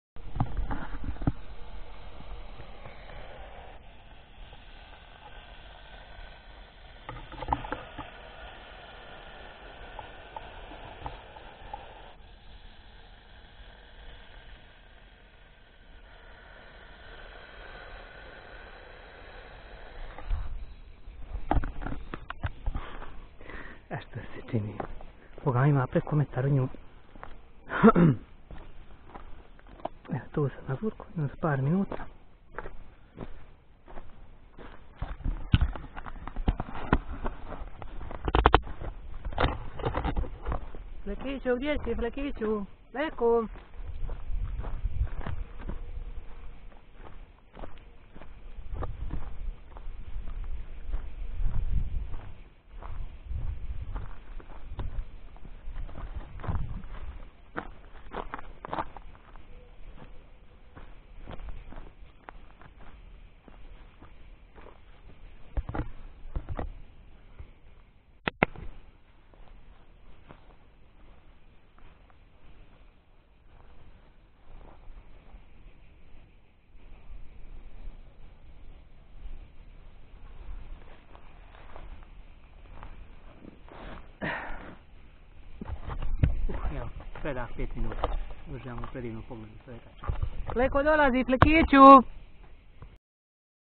Tarp Shelter Overnight On Devesilje Hill - Bez satora 💪👍👊🐕❤ Sa Flekicem Nocenje 03-04. jul 2021.
Zmija morao sam se malo pribliziti-Snimak izbliza 😲😂☺